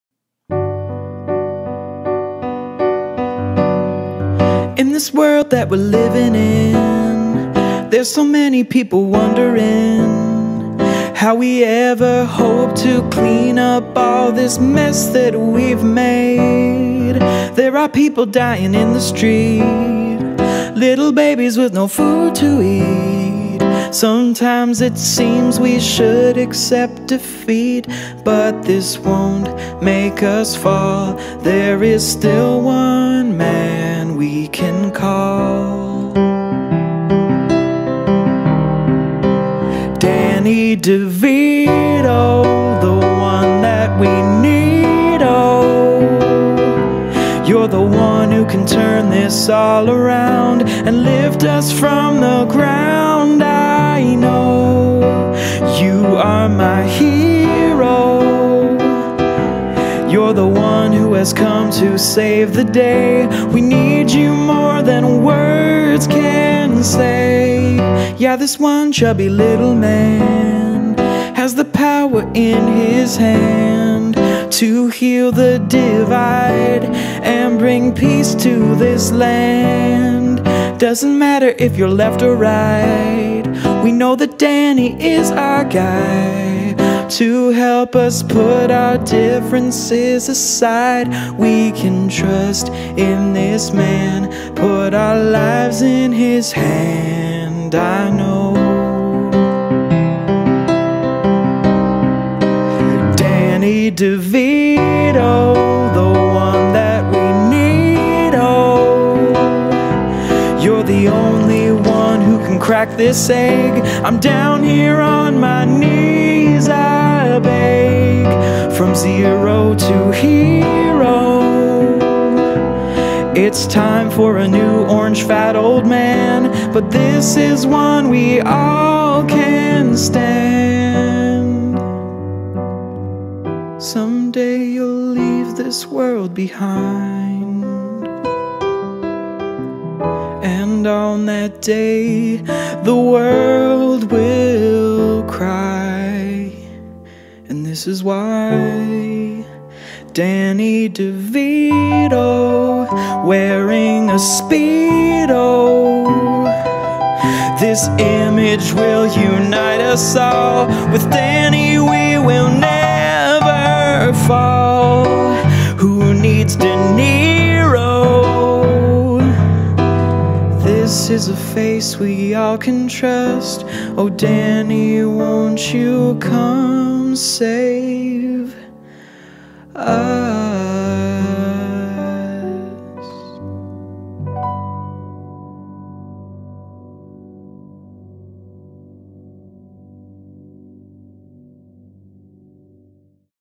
Danny DeVito Tribute
To honor the great Danny DeVito I created this video highlighting some of the great quotes and facts of him.